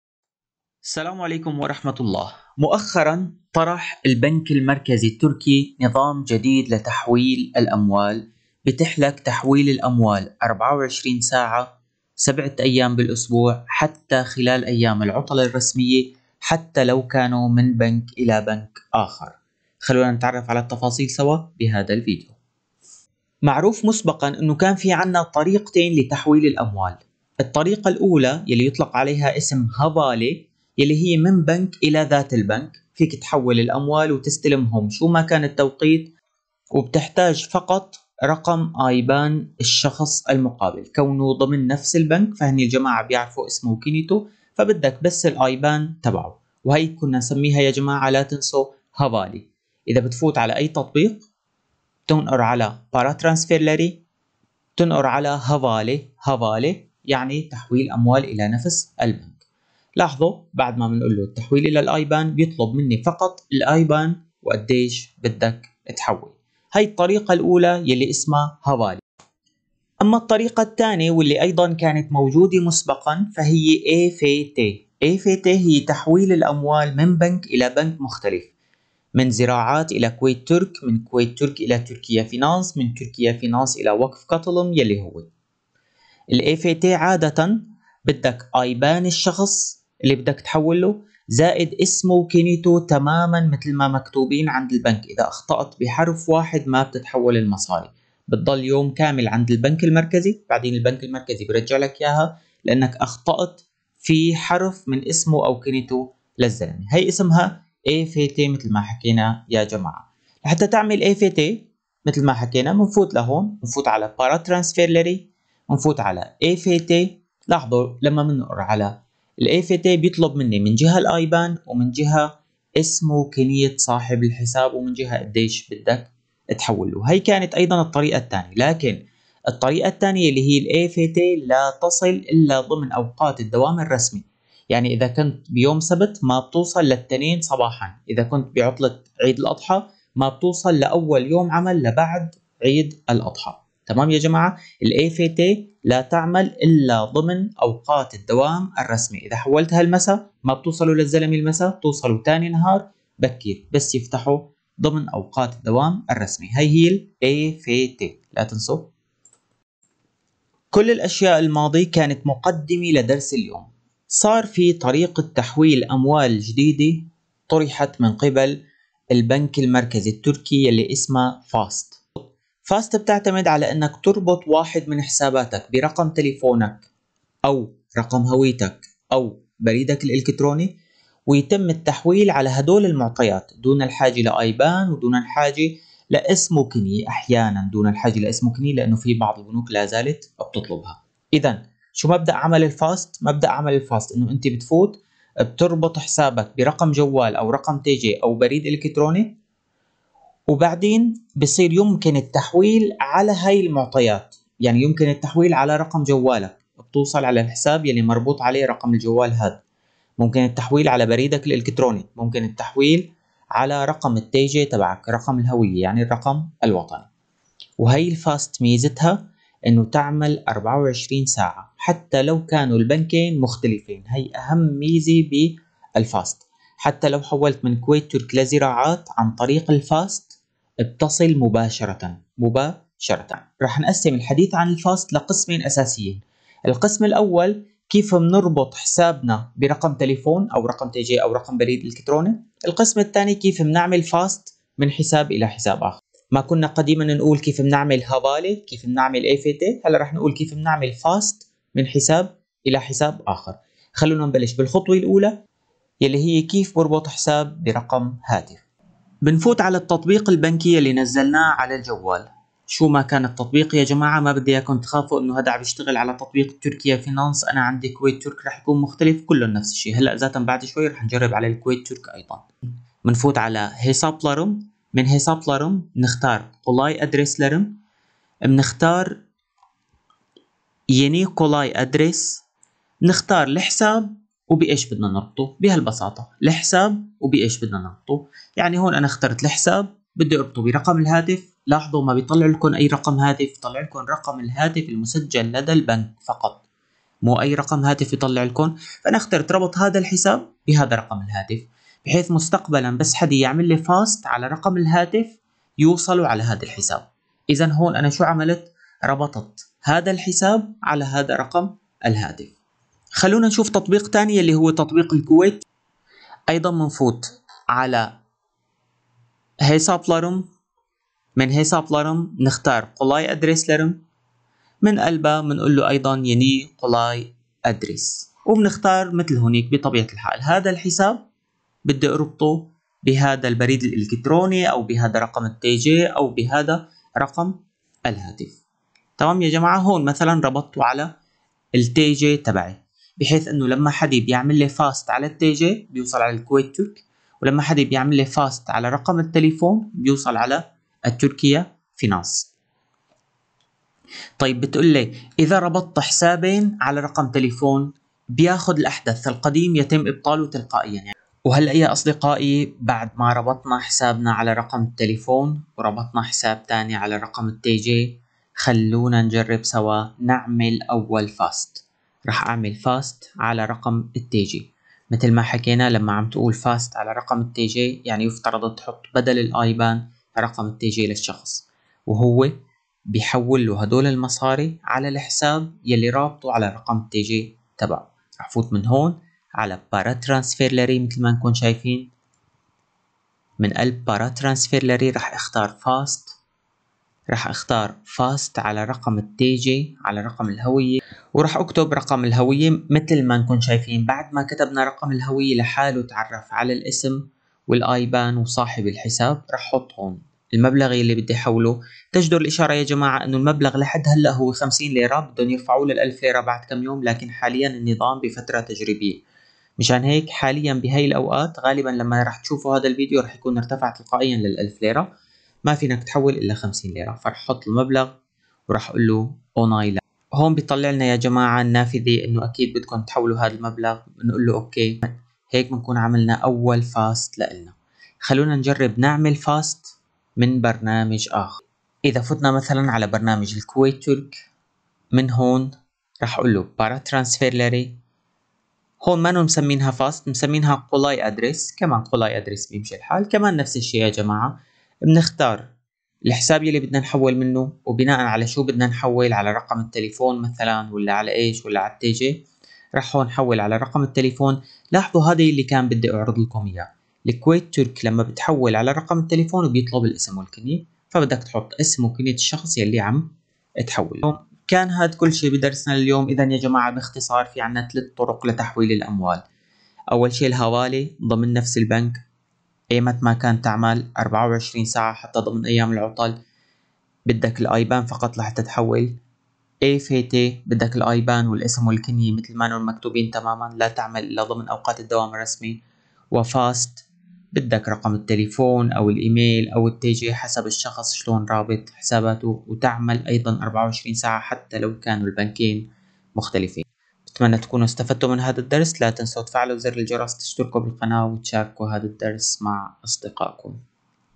نظام تحويل جديد بين البنوك التركية (حوالات بين البنوك المختلفة 24 ساعة)
Kolay Adreslerim , FAST
نظام فاست